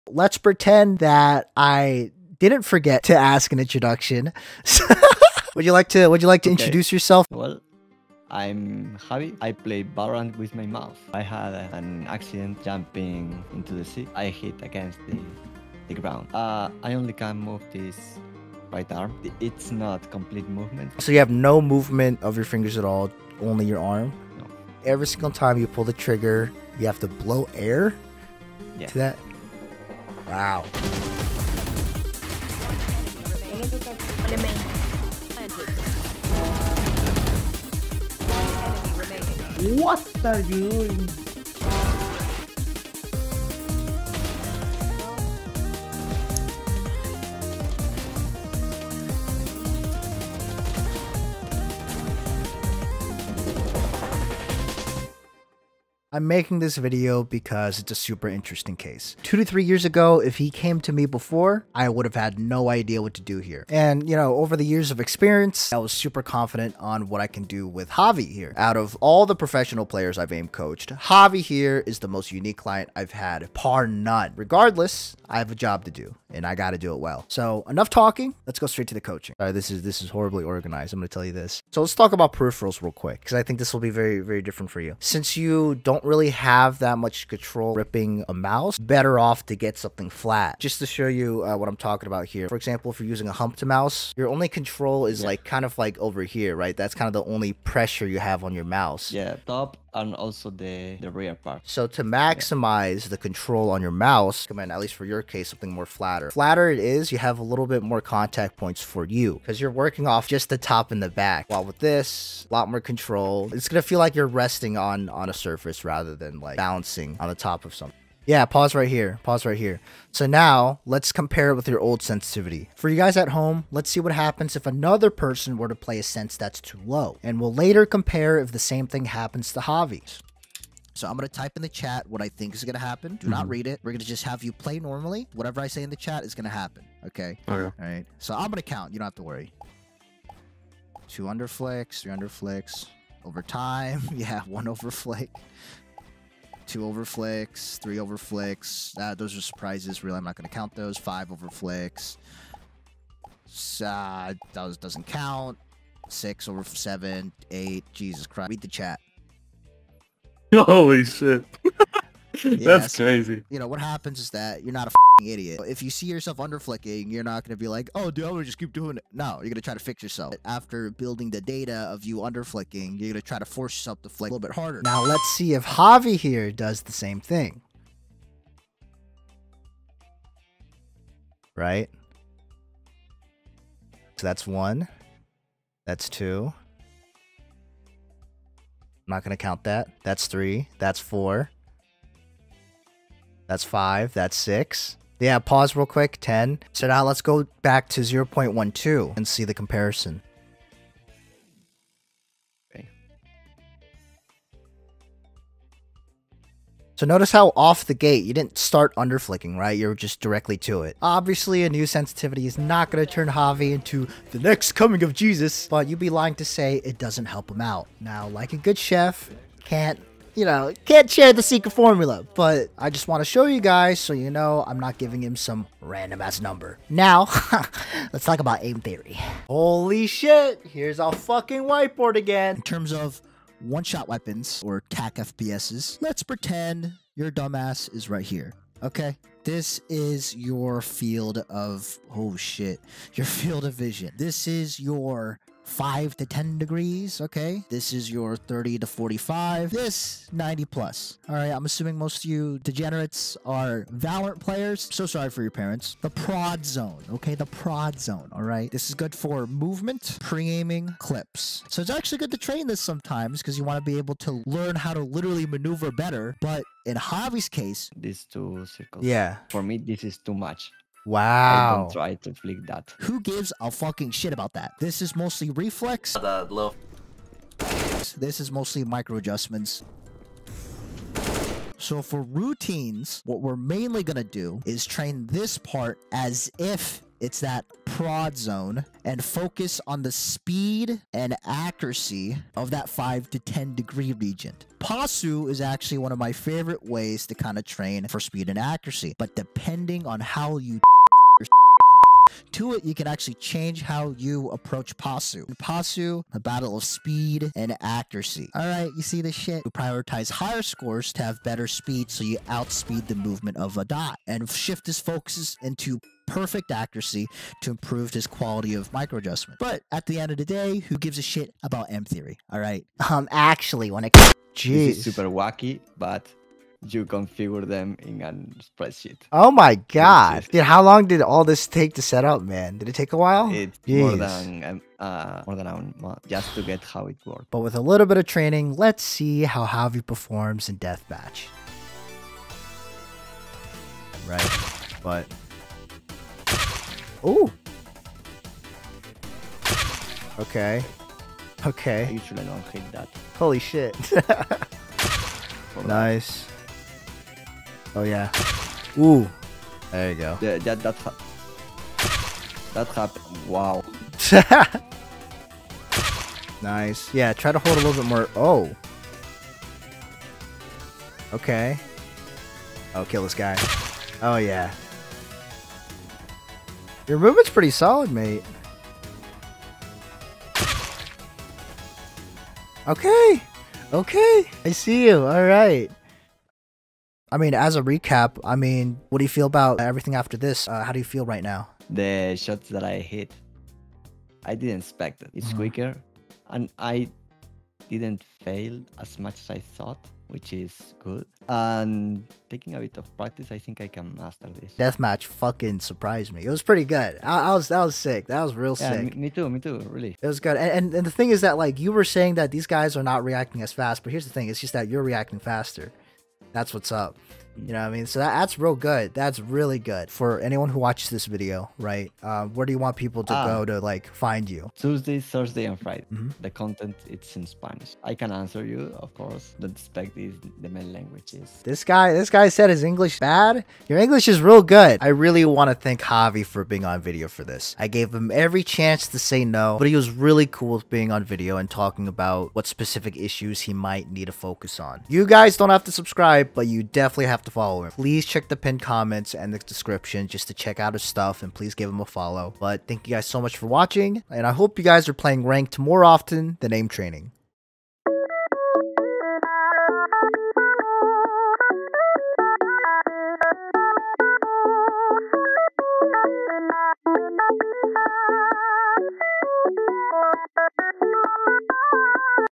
I Aim Coached a Quadriplegic Aimer (He was Insane)
Please give him some love!!!

0:00 Meet Javi
1:19 Peripherals
1:58 Sensitvity
3:49 How I trained Javi
5:44 Results
6:26 Conclusion with Javi